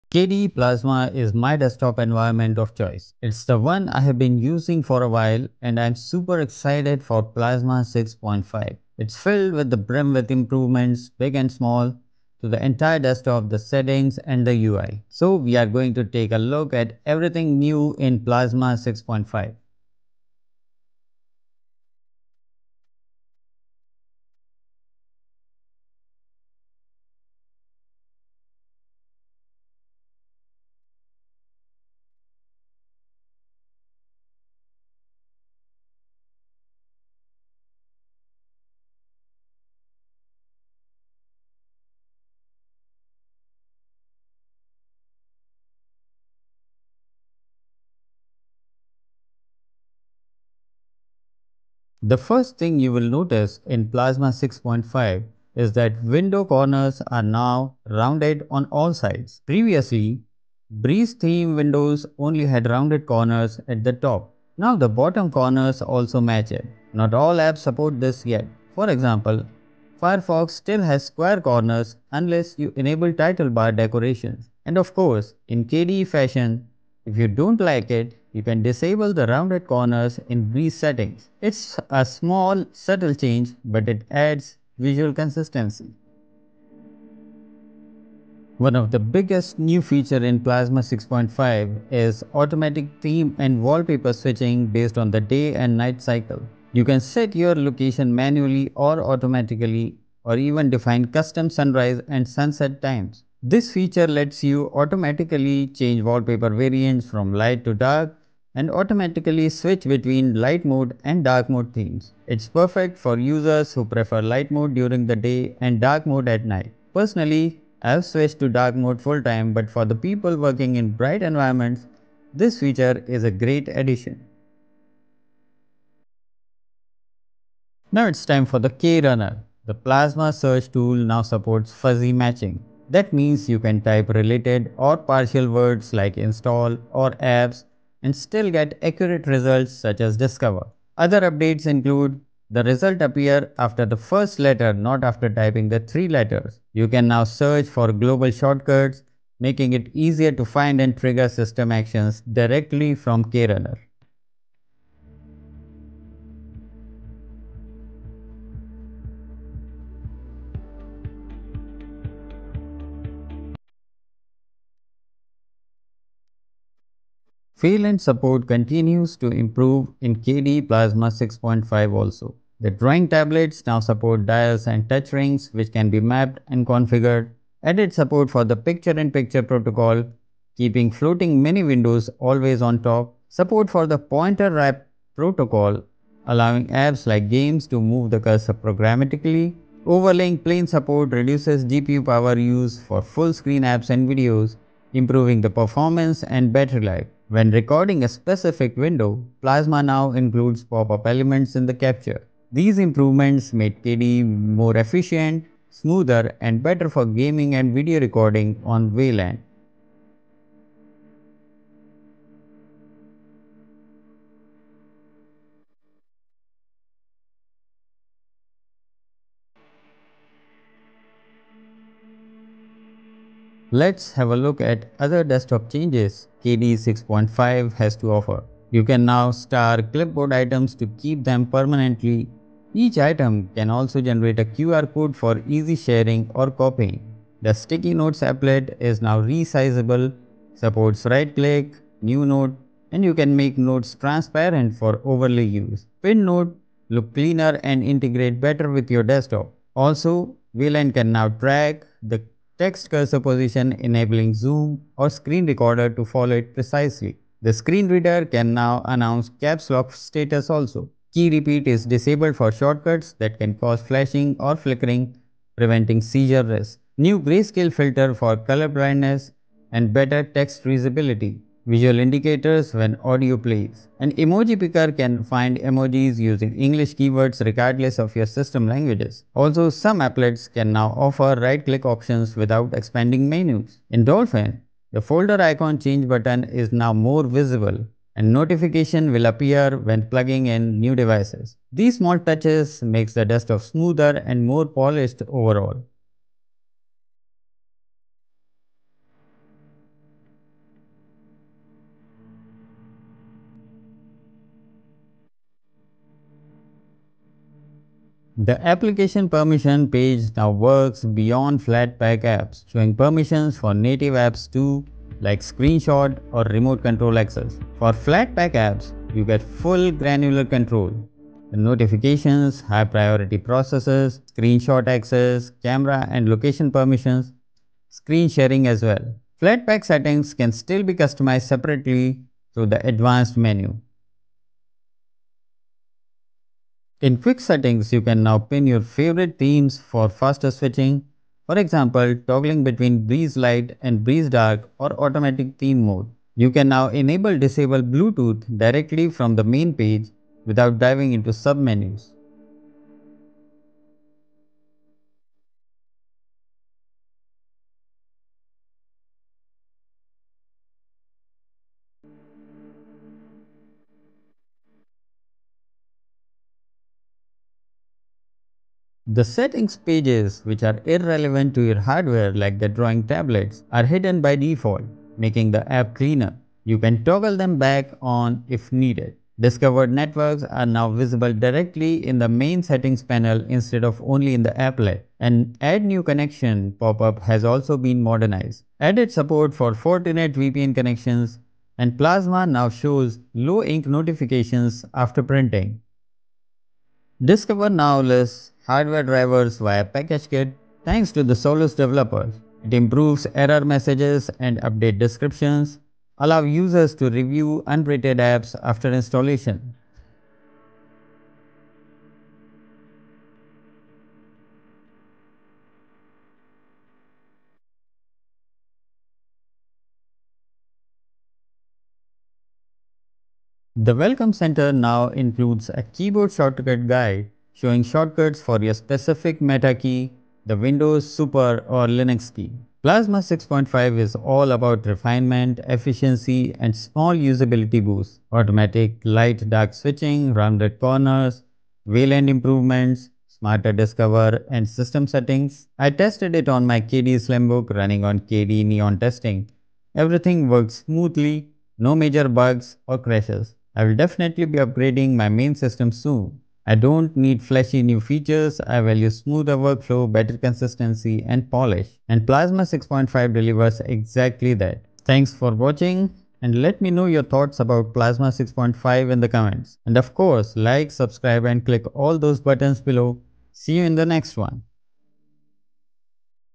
5 Reasons KDE Plasma 6.5 Beats GNOME For BEST Linux Experience
KDE Plasma 6.5 is here — and it’s all about refinement, polish, and smarter workflows!
From rounded corners and automatic light/dark mode to huge Wayland improvements and smarter system settings, this update makes Linux desktops feel smoother and more consistent than ever.

In this video, we take a full tour of KDE Plasma 6.5 — covering every change, big and small. Whether you’re on KDE Neon, Fedora KDE, openSUSE, Arch, or Kubuntu, you’ll see how Plasma 6.5 elevates your desktop experience.

00:00 Intro
00:51 Rounded Corners, Desktop and UI Changes
01:28 Day and Night Cycle
02:07 KRunner Improvements
02:47 Wayland Improvements
03:43 Other Desktop Changes
05:08 System Settings and Apps
            Quick Theme Switching
              Bluetooth
              Hidden Unused Settings
               Wi-Fi
               VPN and Printing
06:37 Discover
           Welcome Center
07:04 Conclusion

💡 Highlights

Rounded corners & resizable sidebars

Automatic light/dark mode switching

Fuzzy search in KRunner

Improved Wayland gaming & recording

Smarter clipboard, sticky notes, and accessibility options

Redesigned System Settings with new app permissions

Discover updates and Welcome Center shortcuts

🔧 Tested On

KDE Neon Testing • Plasma 6.5 • Wayland Session